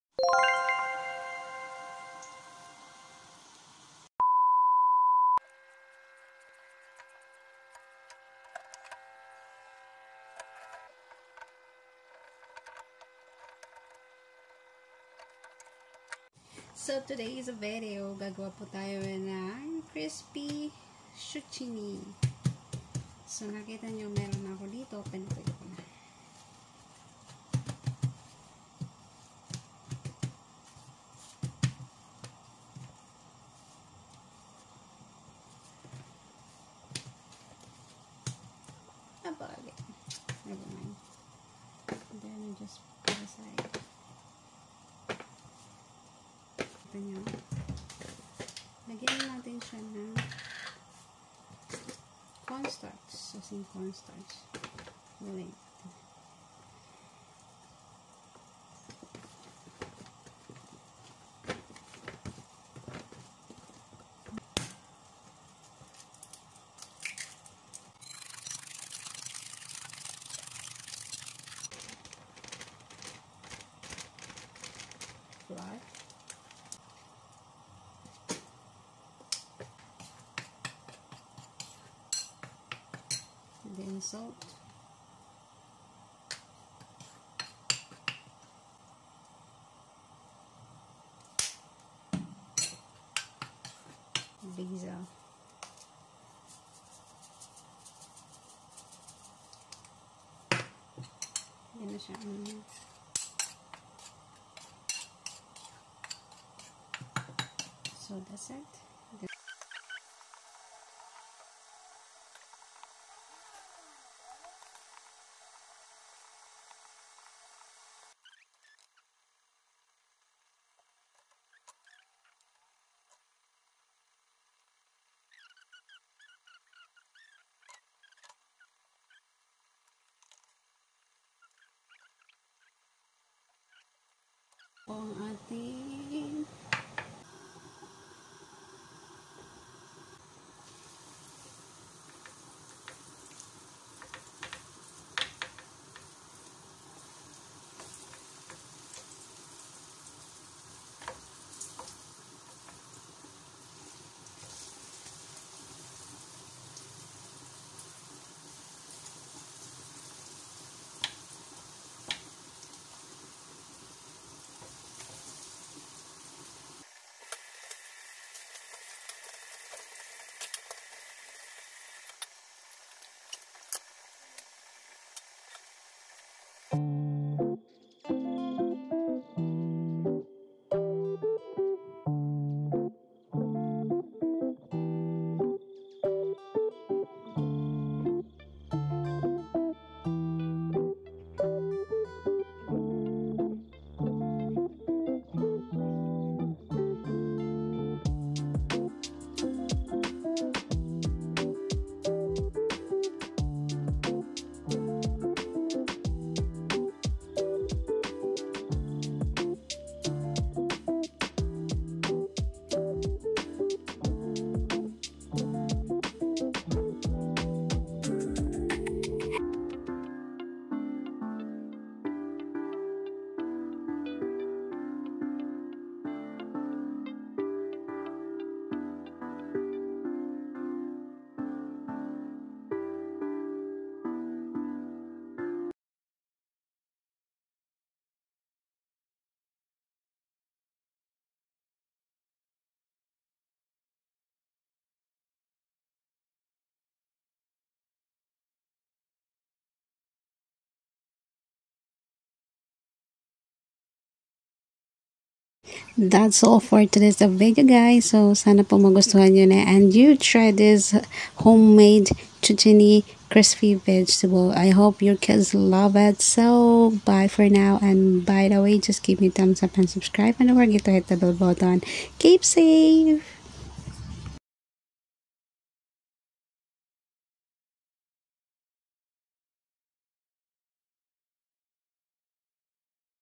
Sawa naba kayo sa ginisang gulay ito ma's simple|| Crispy Zucchini
Ito ung simple pero masarap at magugustuhan  ng mga kids s amga hindi MAHIHILIG sa gulay,  just try at home  pa maparami ang kain NG mga anak natin  CRISPY ZUCCHINI